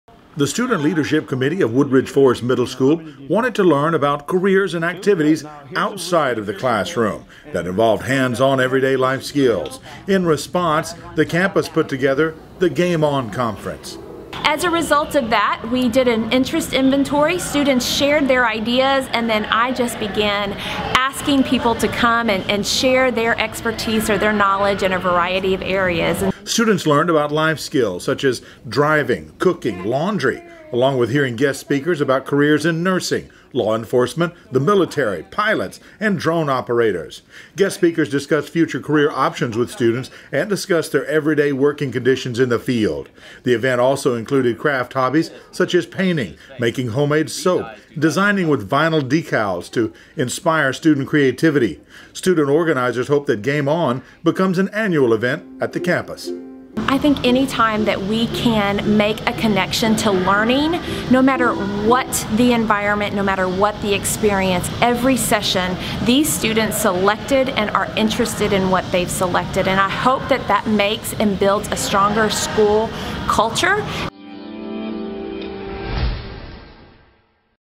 Game On at Woodridge Forest Middle School
Students got a chance to see what a day in the life was like for today professionals.